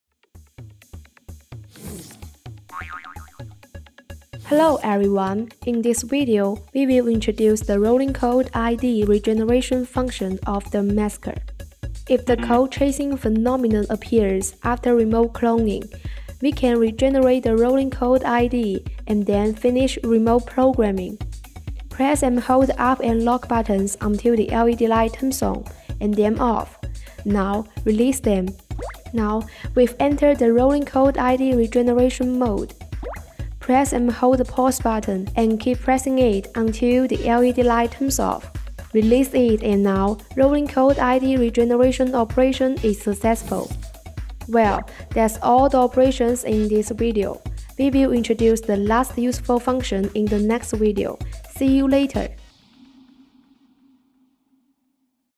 How to Regenerate Rolling Code ID | Xhorse Masker Garage Remote
Xhorse MASKER Garage Remote : Code chasing phenomenon can be easily solved

Click the video to learn how to regenerate rolling code ID